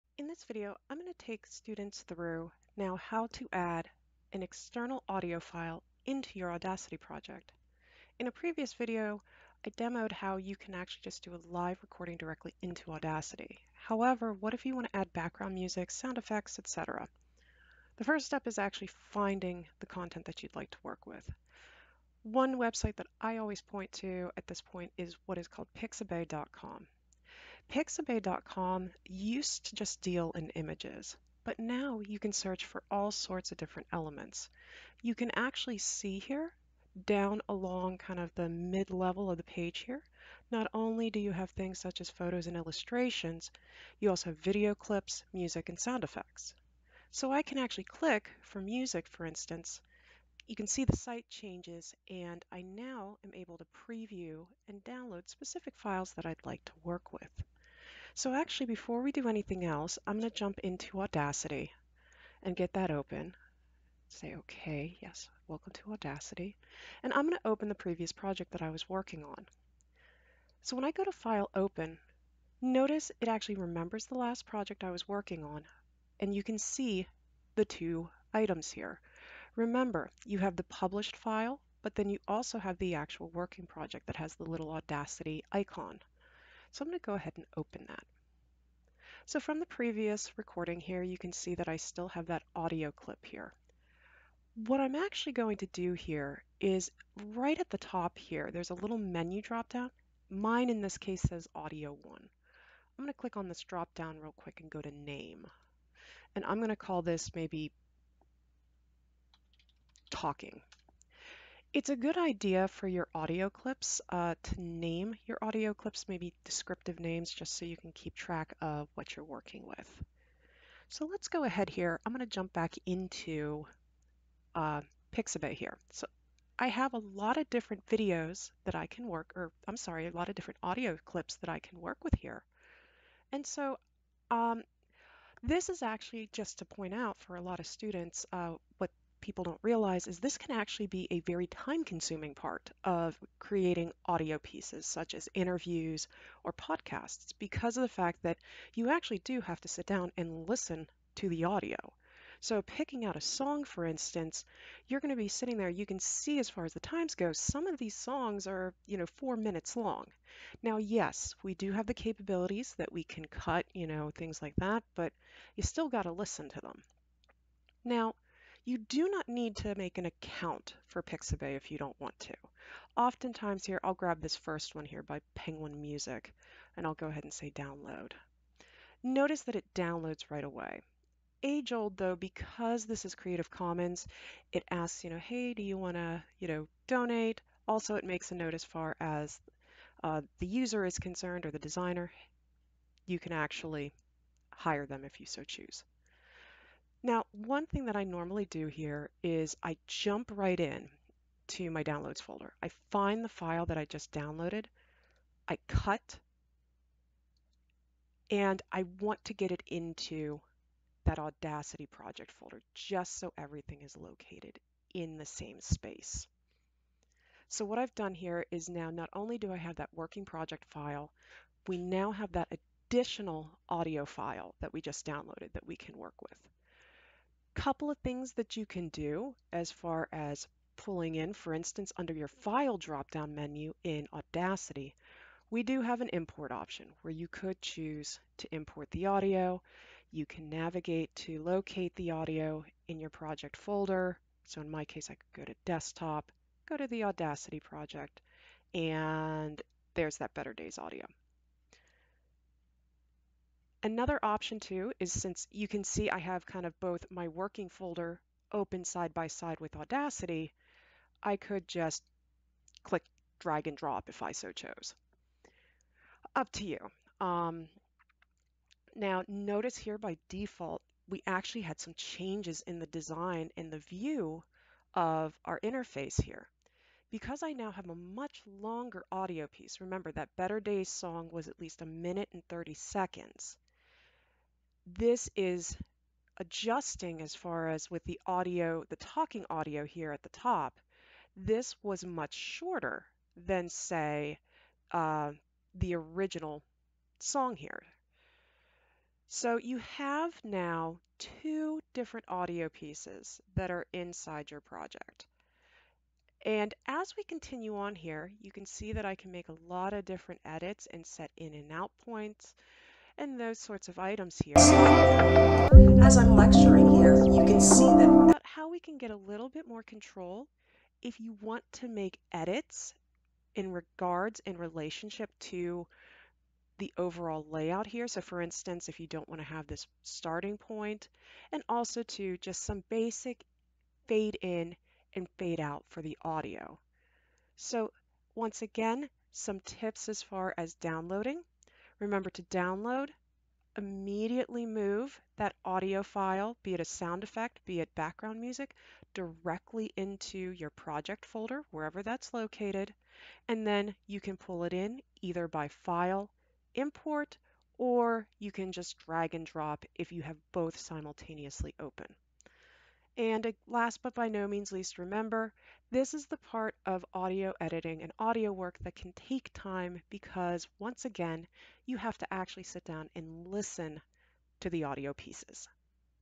Audacity - Importing External Audio Files into a Project
This short how-to shows how to download, organize, and import an external audio piece into an existing Audacity project.